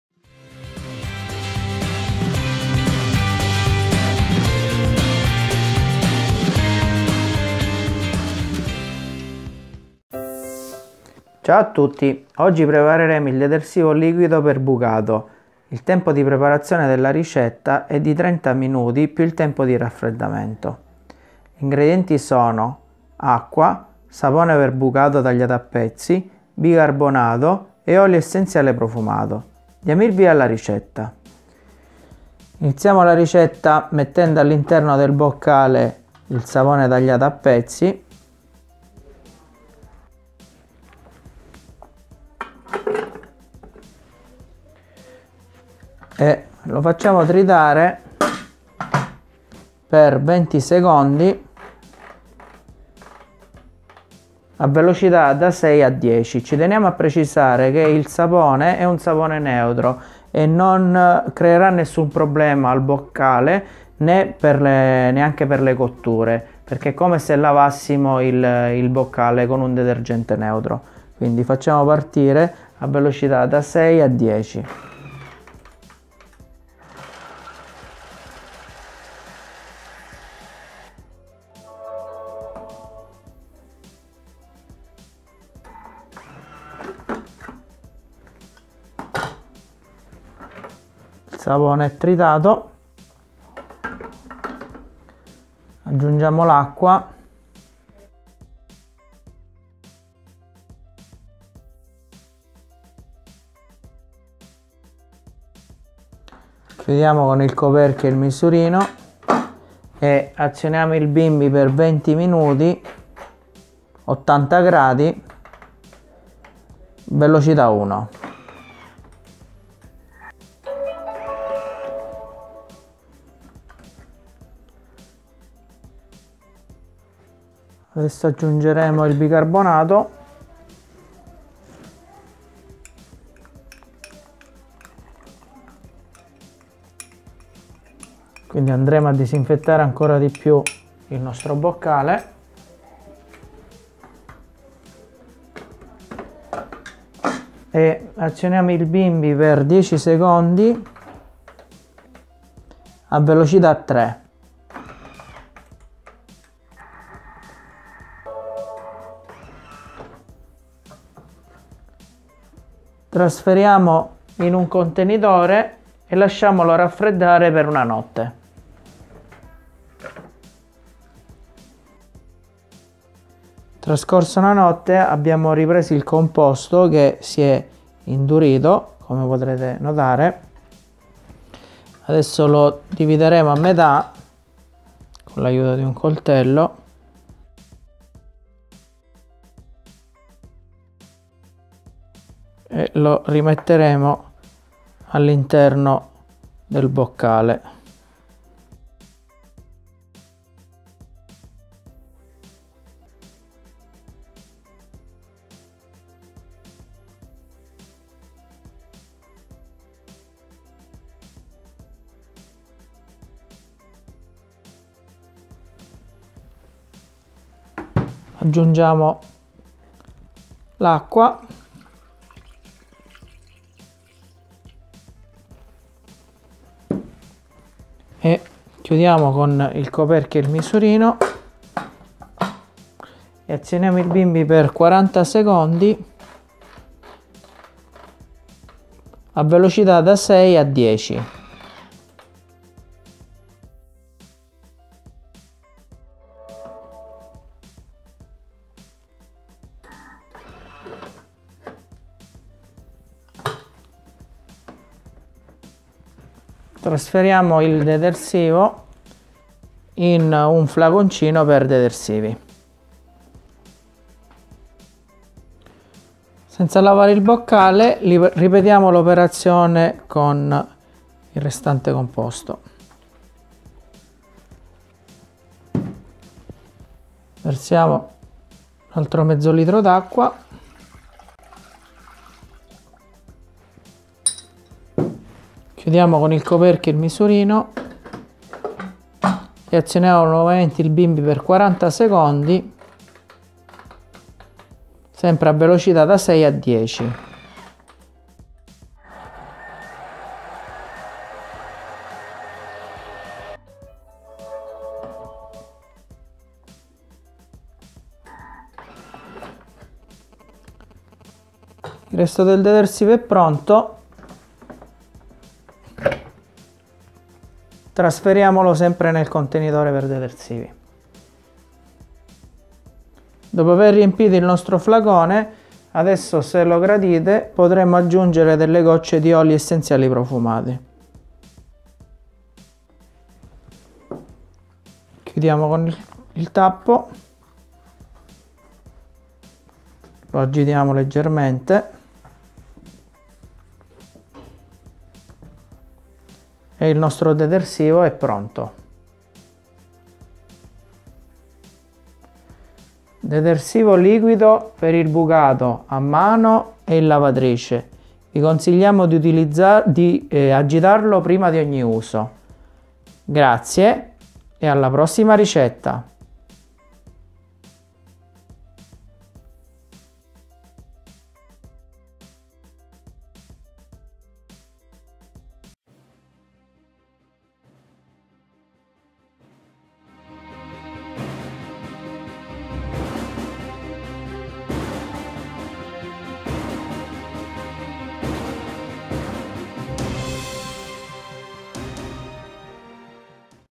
Detersivo per il bucato a mano o in lavatrice bimby per TM6 e TM5
Attenzione queste dosi sono solo per il bimby TM6 e TM5
Per il bimby TM31 bisognerà dimezzare le dosi mantenendo invariati i tempi e le velocità

Ingredienti:
2 litri di acqua  + 1 litro da aggiungere dopo
100 g di sapone per bucato a pezzetti
50 g di bicarbonato
15 gocce di olio essenziale profumato  (facoltativo)
1 Flaconcino dalla capacità di 5 litri